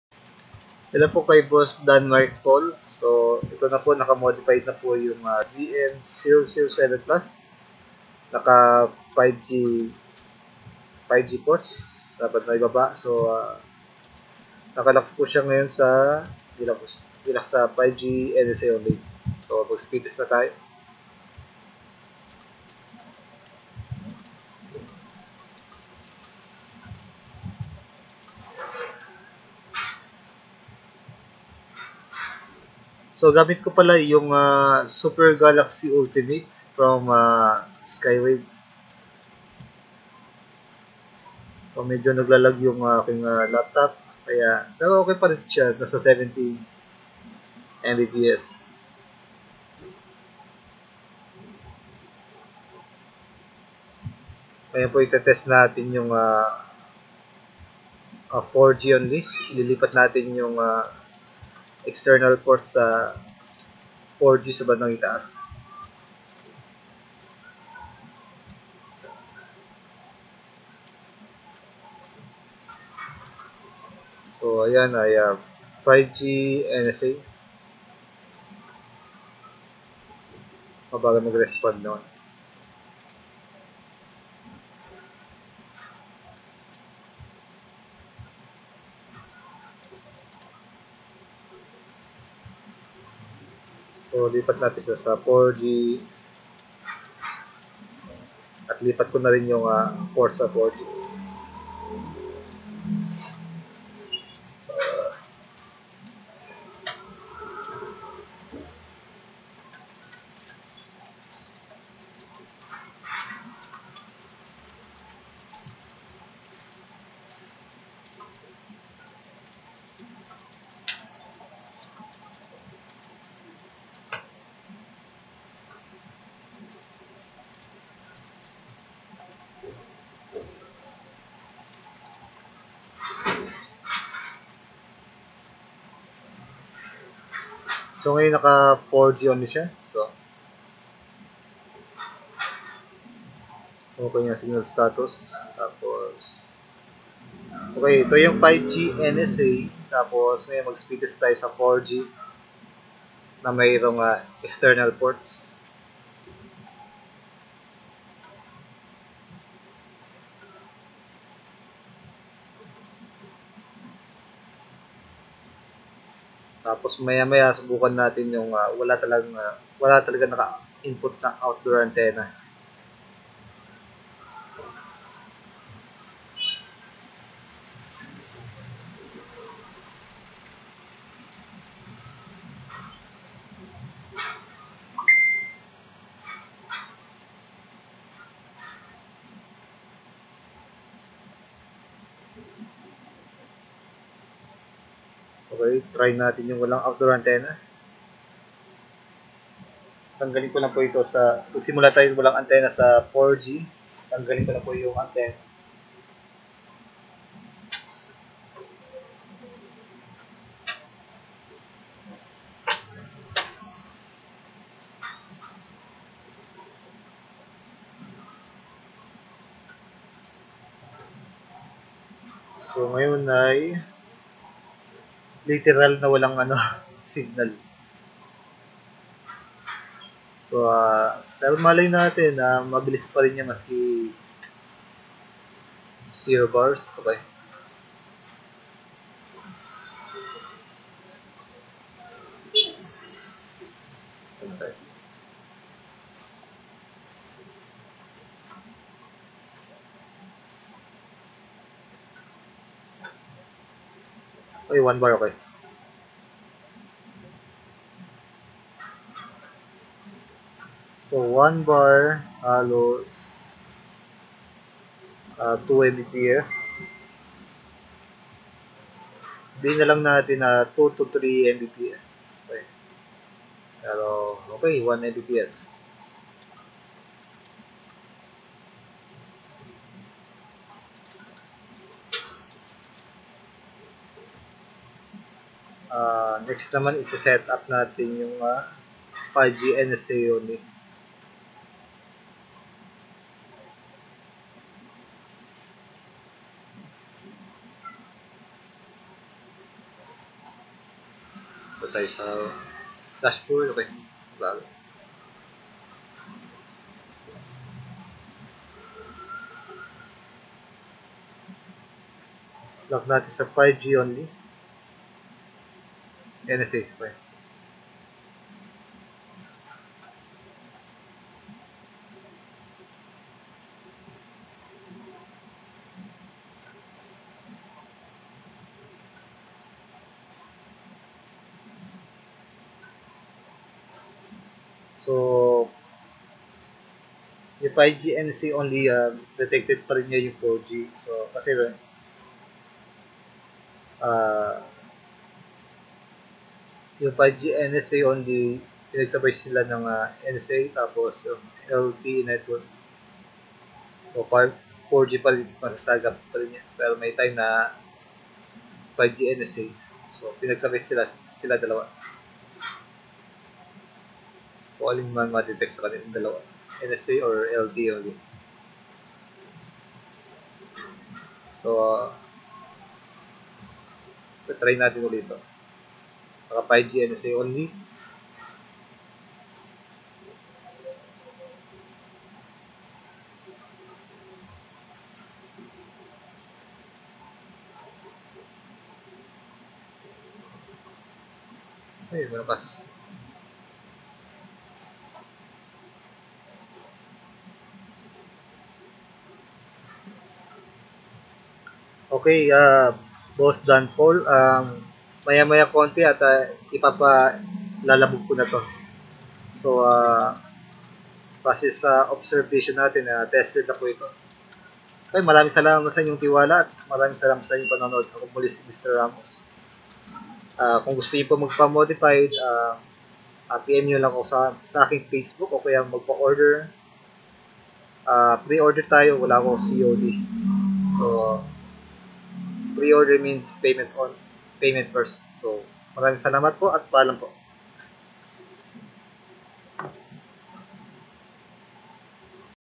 VN007+ to boss DANMARK
Thank you sa tiwala boss Danmark paul

pwede kayo bumili sa shopee link na ito tested at trusted iyan MJL
siya nag bibigay ng Root password ng VN007+

antenna use
super galaxy ultimate antenna